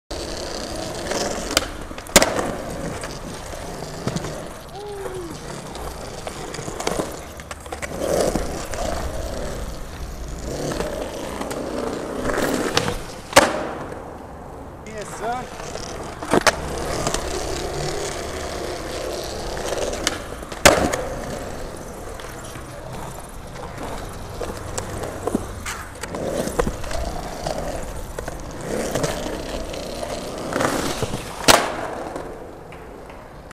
Clip Of The Day (COTD) 8
Hizzle is a line machine, id save these for a montage but he could probably do 2 more right quick seeing as these were both first and second try.